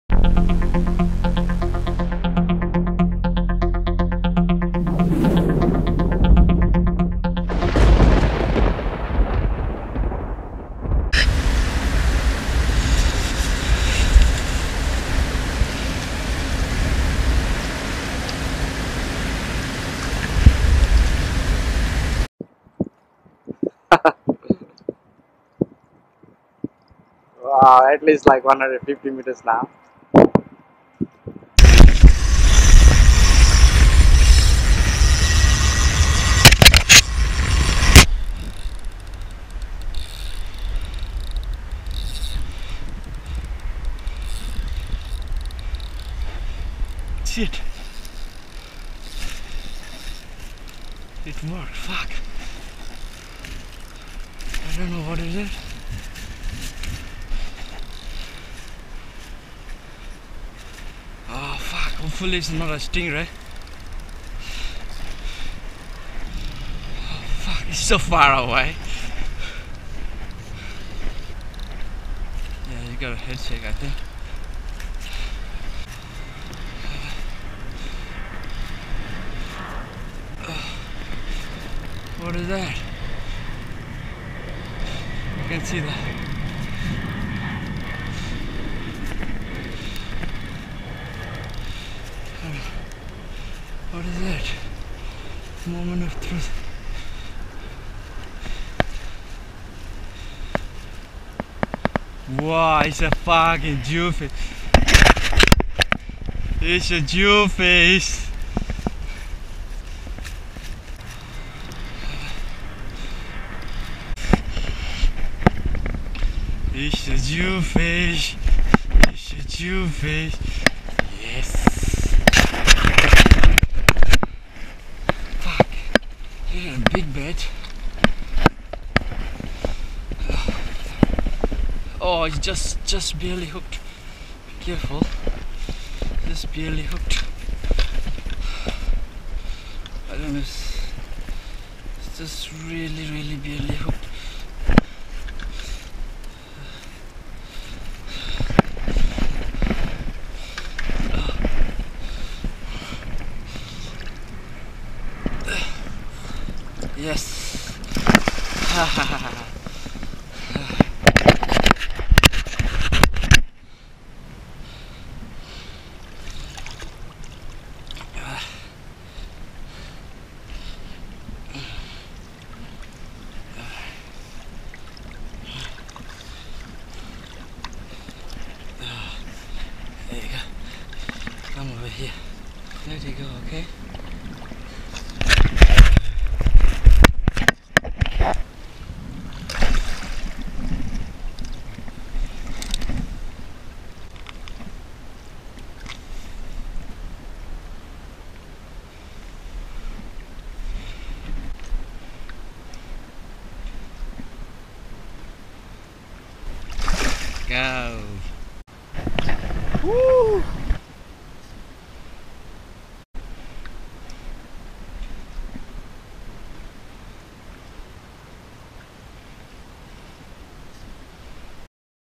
Ballooning Rig : Jewfish Part - 2
Fishing for jewfish became very difficult in winter as a land-based angler. As shallow water became colder and fish started to move deep waters,  I needed to adapt and came out with ballooning my live bait to deeper holes with right wind. While ballooning is a game of passions... but did paid off. Hope your guys will enjoy the my short video.. thanks everyone for watching and sharing.. :)